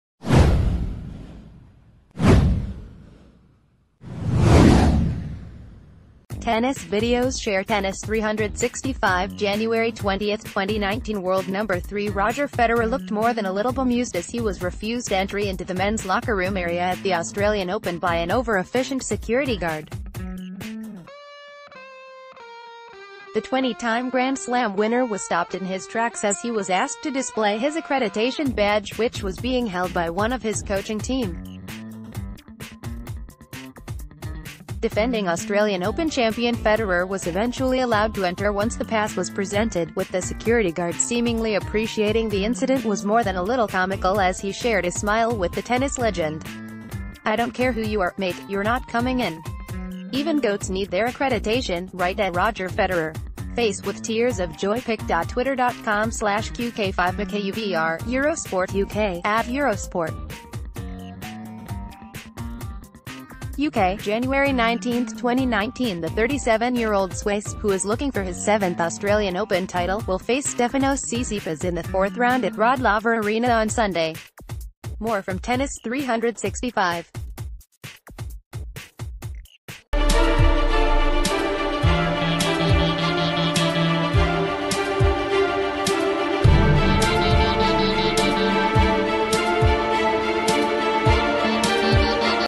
WATCH: Roger Federer denied entry to men's locker room at Australian Open - Tennis365.com
Tennis Videos Share Tennis365 January 20, 2019 World No 3 Roger Federer looked more than a little bemused as he was refused entry into the men’s locker room area at the Australian Open by an over-efficient security guard. The 20-time Grand Slam winner was stopped in his tracks as he was asked to display his accreditation badge, which was being held by one of his coaching team. Defending Australian Open champion Federer was eventually allowed to enter once the pass was presented, with the securit...